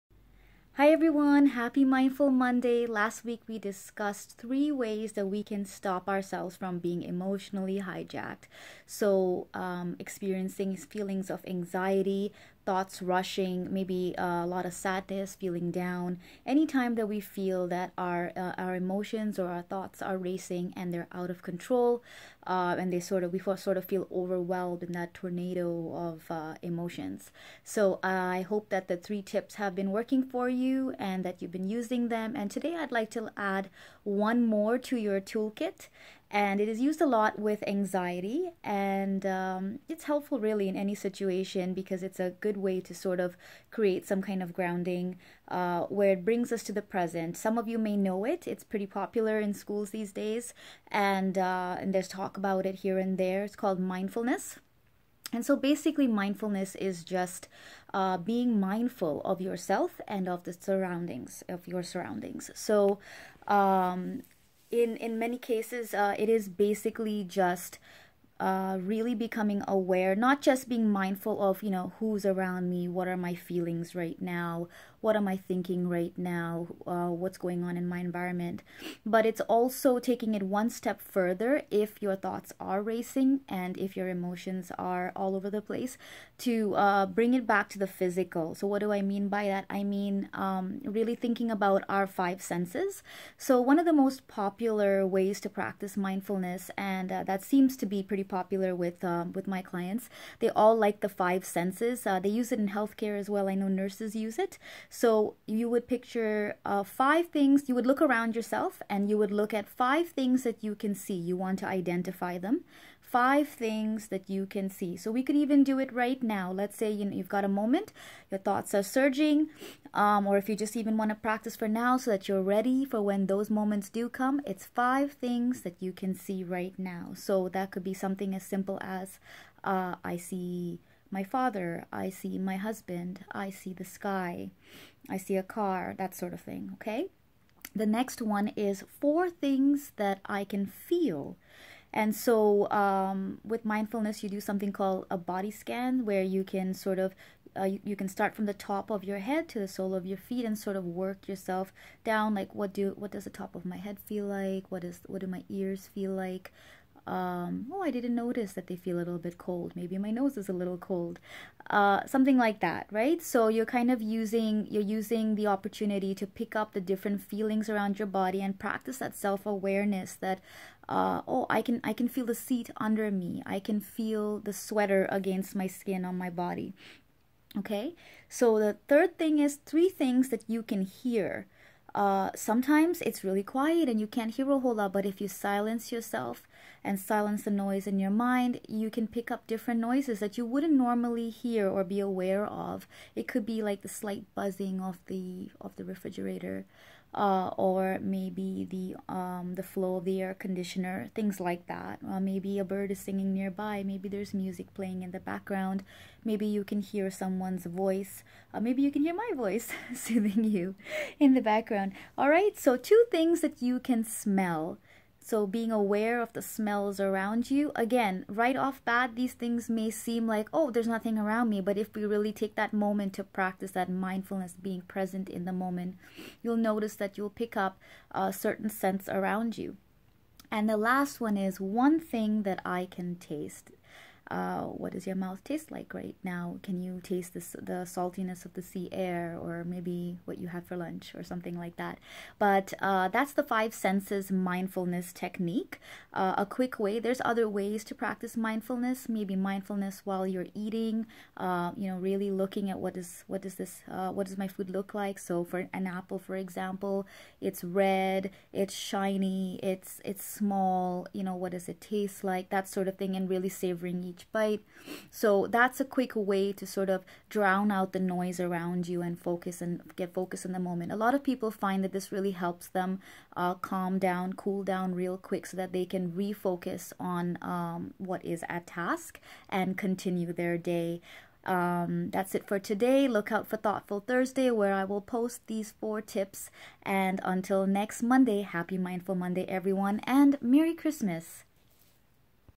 Mindful Monday 11 - Use Mindfulness for Anxiety/Stress/Distress
Merry Christmas everyone!! I am adding 1 MORE technique to your toolkit this week! Follow along, take notes for quick reference later, or bookmark this video as an aid for when you need help to walked through it.